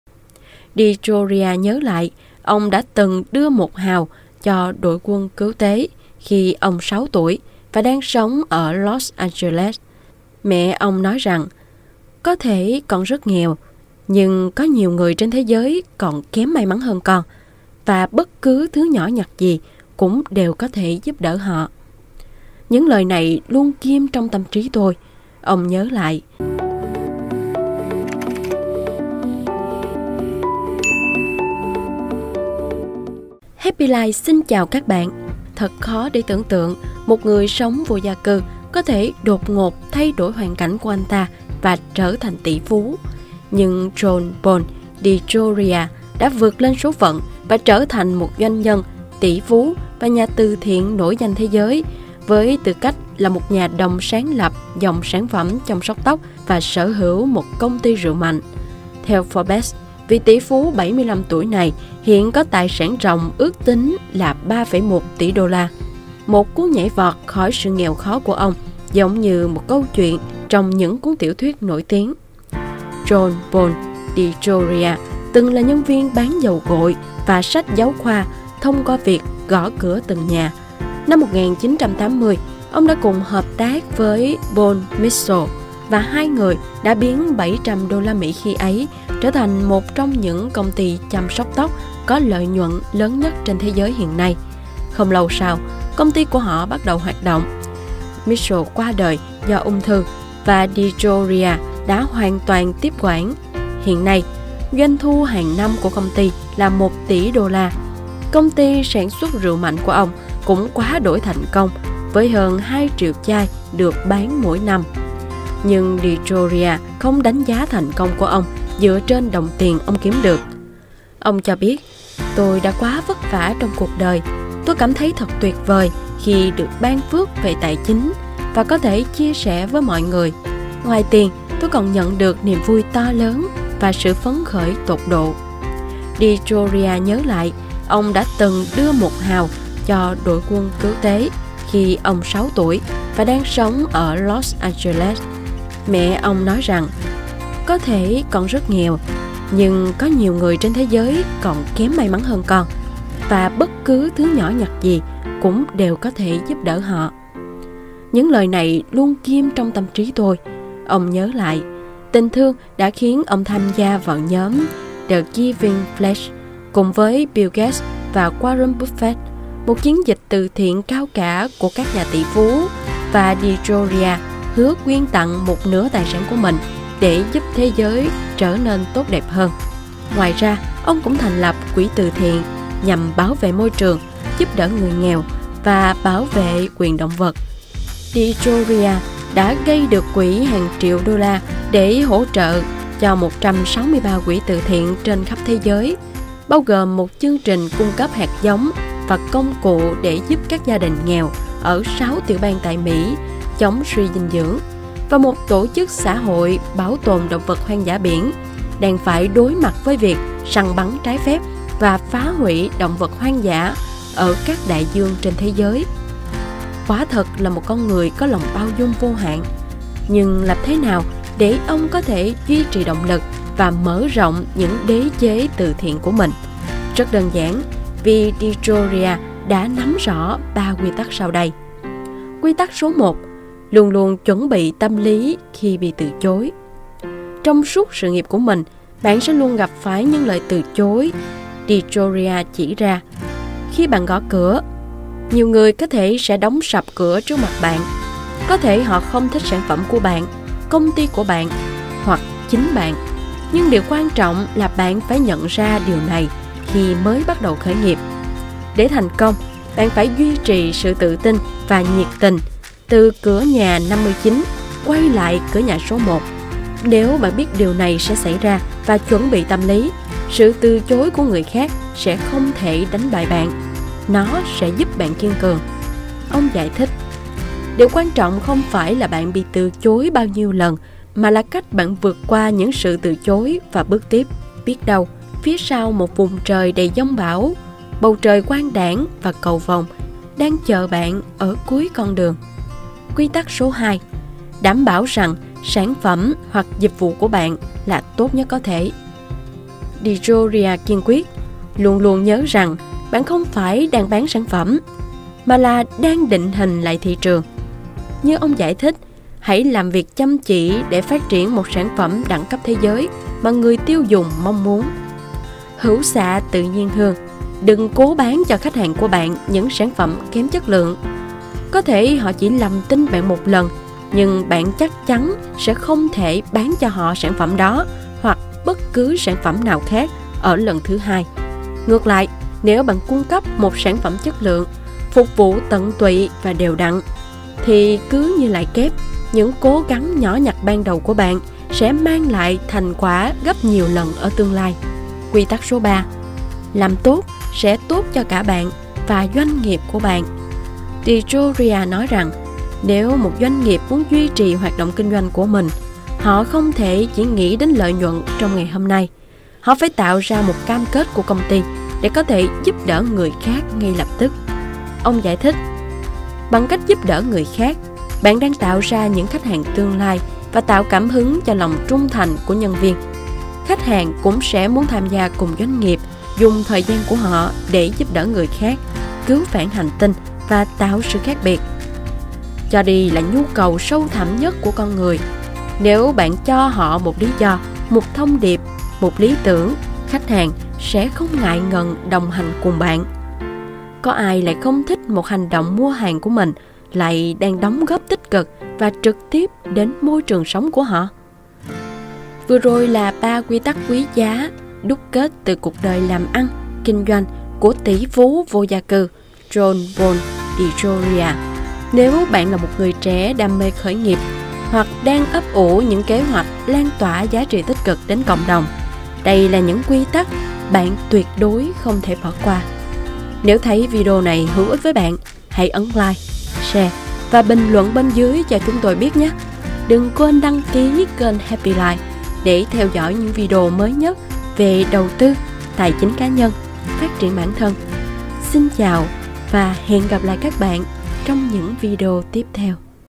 "TỶ PHÚ VÔ GIA CƯ" JOHN PAUL DEJORIA VÀ 3 QUY TẮC THÀNH CÔNG VÔ GIÁ
Thật khó để tưởng tượng một người sống vô gia cư có thể đột ngột thay đổi hoàn cảnh của anh ta và trở thành tỷ phú.

Nhưng John Paul DeJoria đã vượt lên số phận và trở thành một doanh nhân, tỷ phú và nhà từ thiện nổi danh thế giới với tư cách là của một nhà đồng sáng lập dòng sản phẩm chăm sóc tóc và sở hữu một công ty rượu mạnh.

Theo Forbes, vị tỷ phú 75 tuổi này hiện có tài sản ròng ước tính là 3,1 tỷ đô la. Một cú nhảy vọt khỏi sự nghèo khó của ông giống như một câu chuyện trong những cuốn tiểu thuyết nổi tiếng.

⭕️ Tham gia Cộng đồng Happy Live - Đầu tư tài chính & Thịnh vượng tại